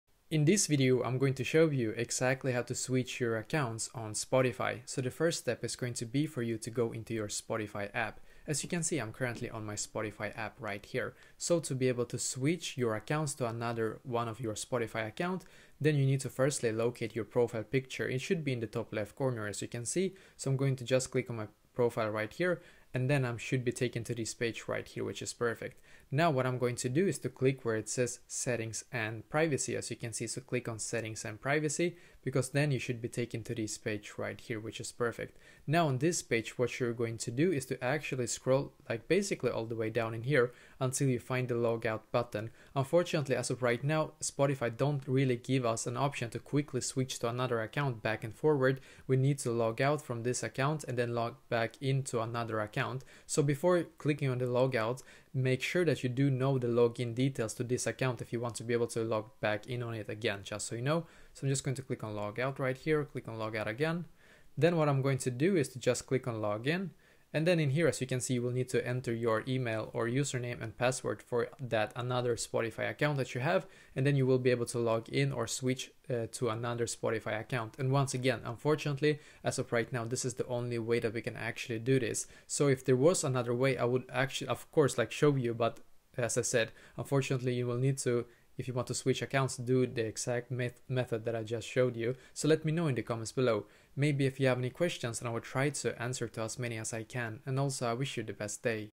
How To Switch Accounts in Spotify (2025)
If you want to be able to switch accounts in Spotify, then this video will be perfect for you!